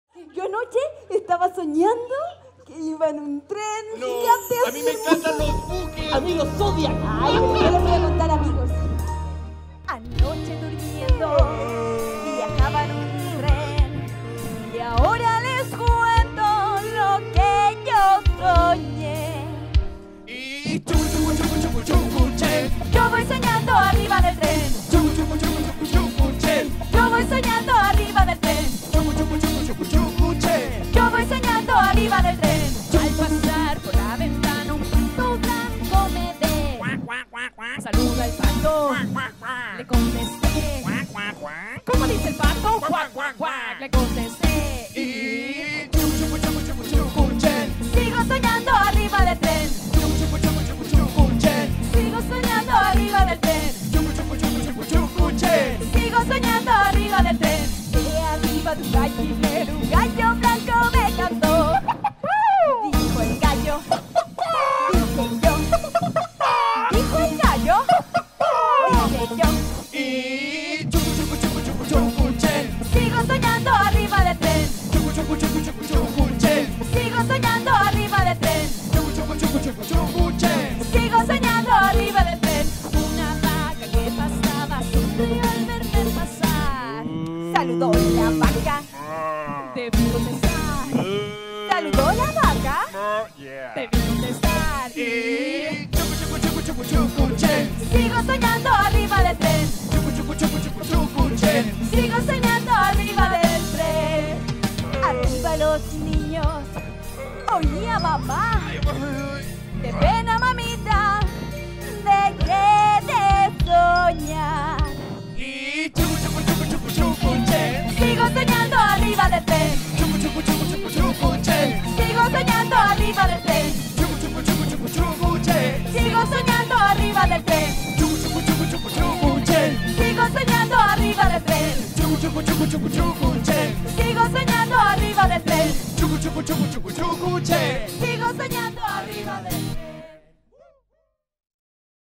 Soñando - ExploraColores en Robinson Crusoe
Un nuevo video de la visita de los ExploraColores al archipiélago de Juan Fernández